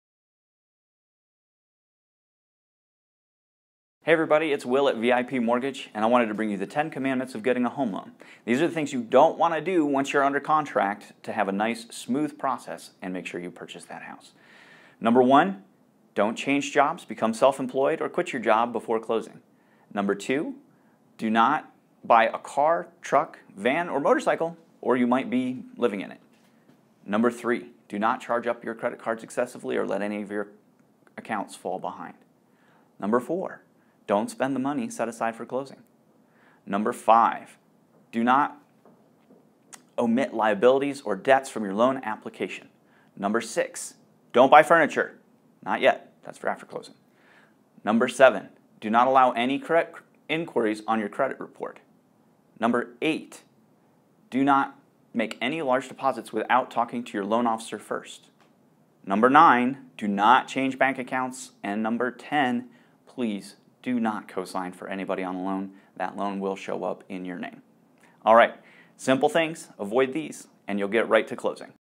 10 Commandments of getting to the closing table TeamSecundy - RE/MAX properties Inc.
10 Commandments of getting to the closing table
Things you should never do once you are Under Contract
TeamSecundy - RE/MAX Properties Inc.